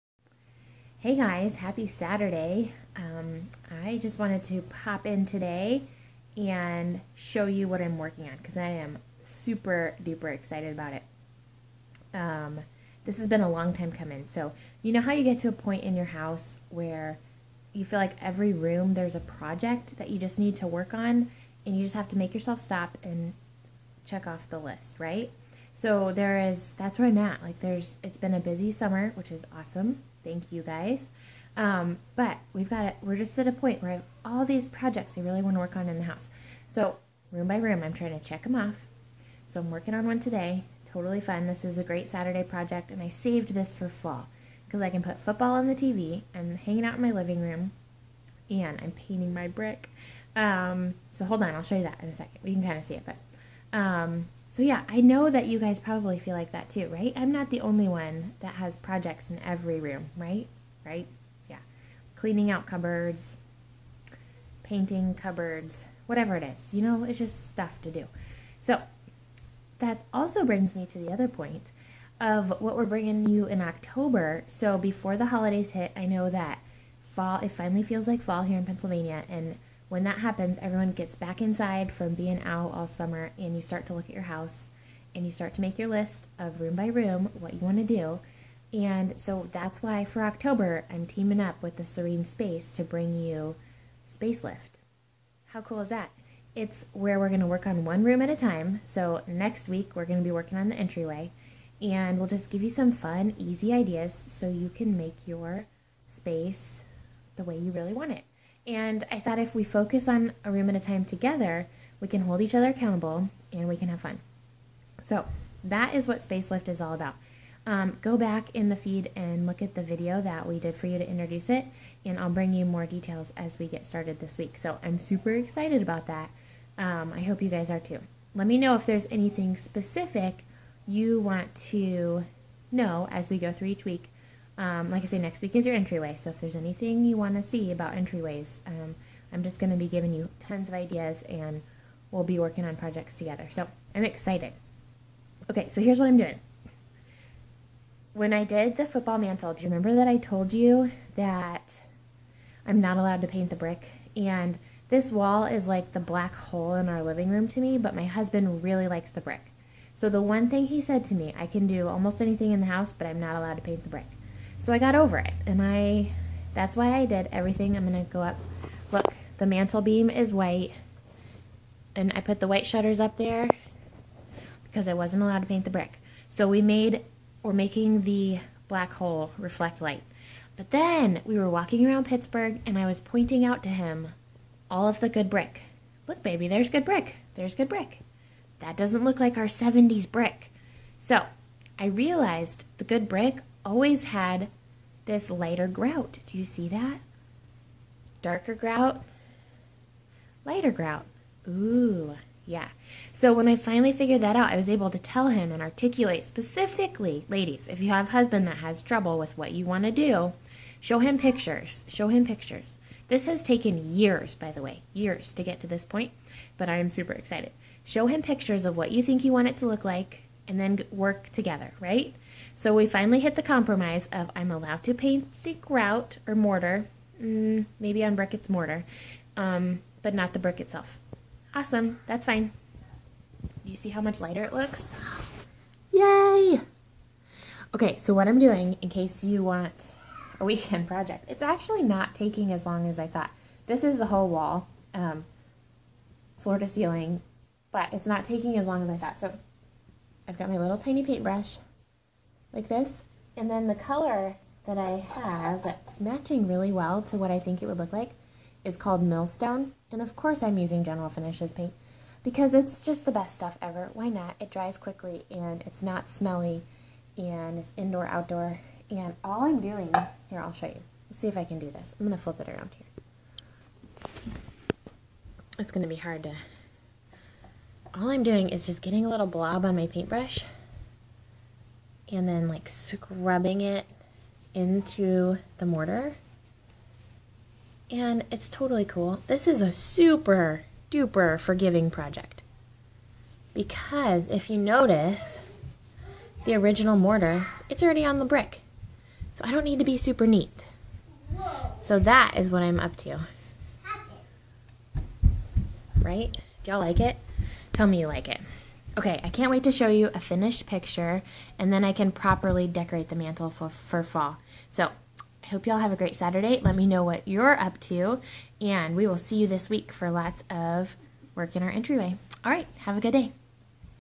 Updating Brick by Painting Grout
Updating an old brick fireplace by painting the grout a lighter color.  By Jenni of Roots and Wings Furniture.